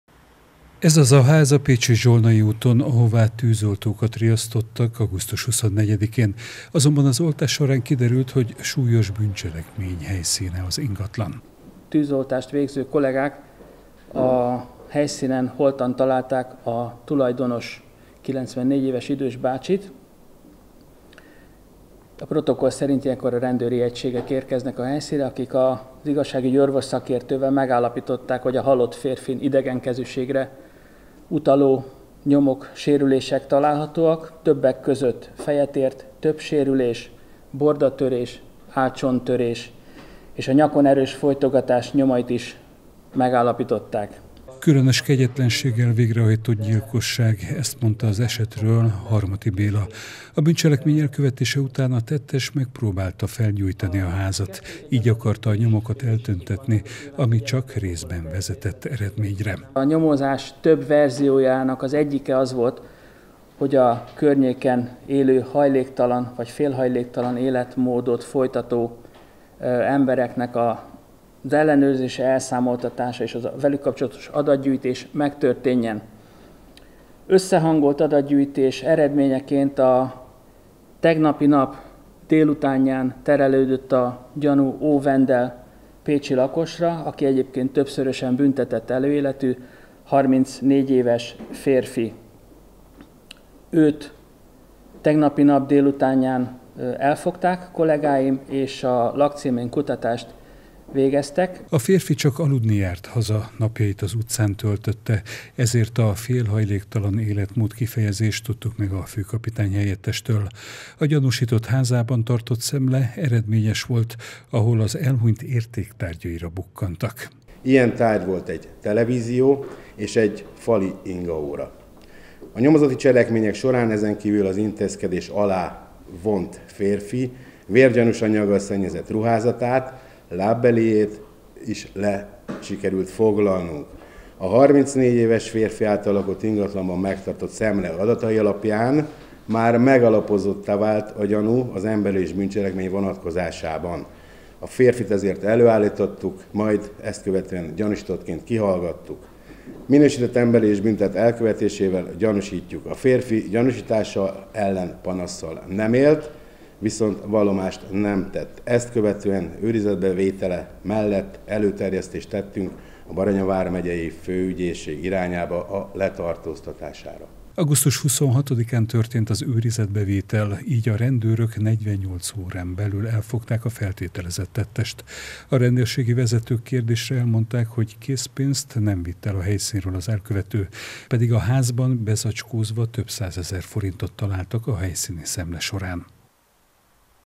KÉT NAP ALATT ELFOGTÁK A GYILKOST
Tűzesetnek indult, végül gyilkossági ügy lett abból az esetből, amiről szombaton értesítették a hatóságokat Pécsen. A lángok oltása során merült fel a gyanú, hogy a Zsolnay úton található házban emberölés történt. A rendőrök végül 48 óra alatt megtalálták a feltételezett tettest.